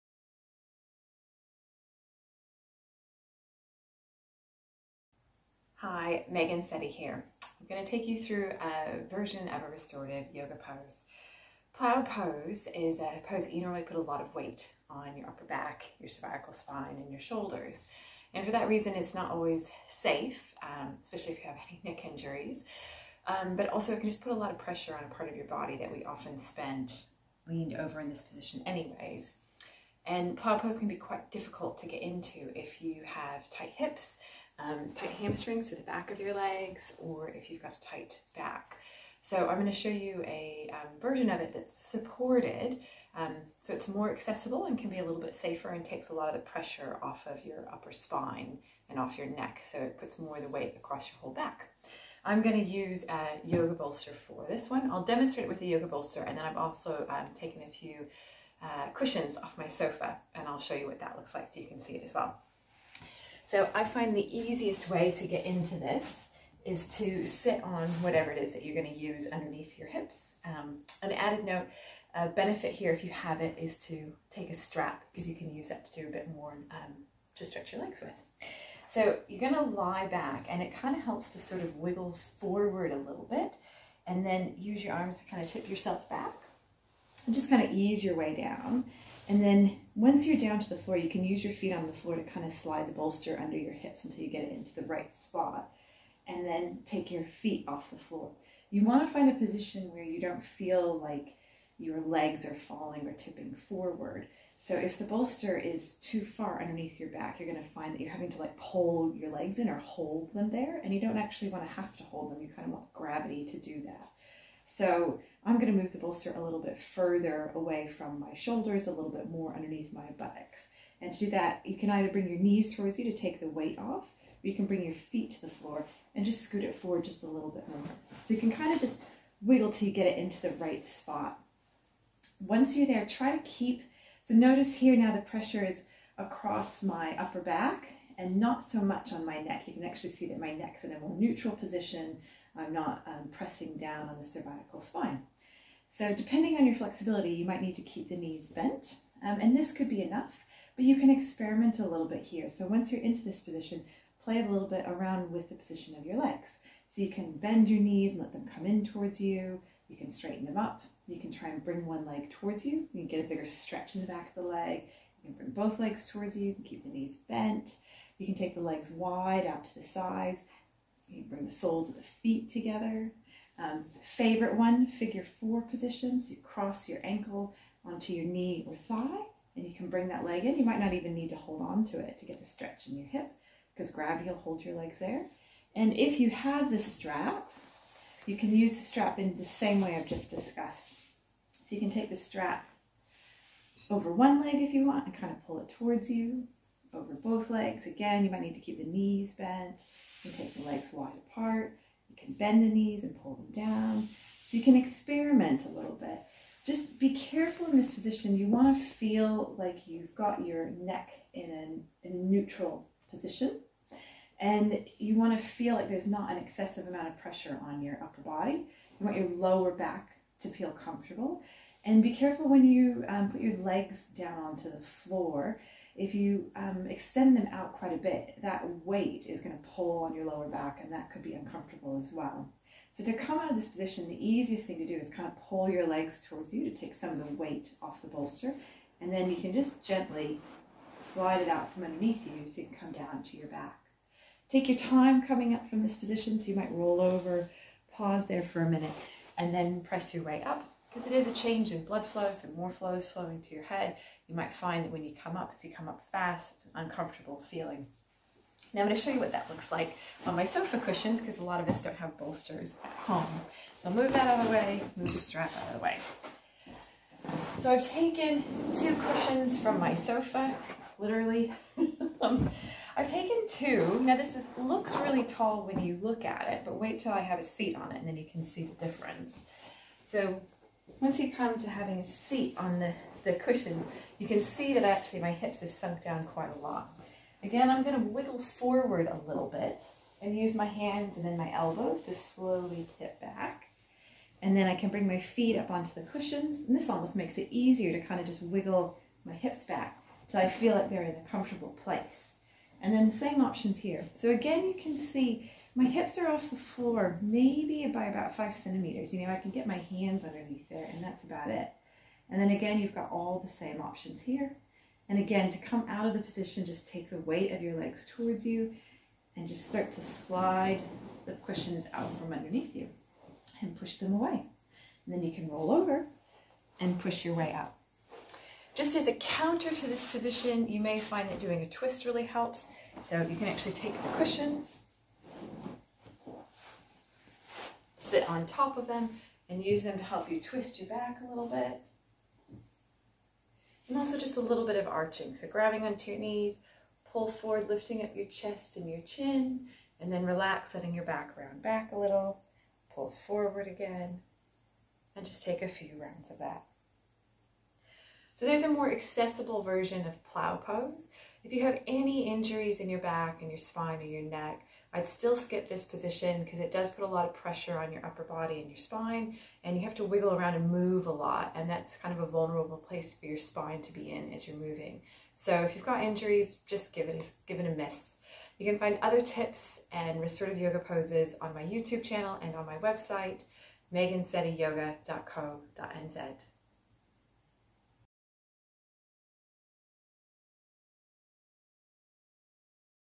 How to do an adapted and supported version of plough pose (halasana)
This video goes through the basic steps of an adapted version of plough pose that doesn't put pressure on the neck but still gives you the benefits of relaxation and stretching the hips, back and hamstrings.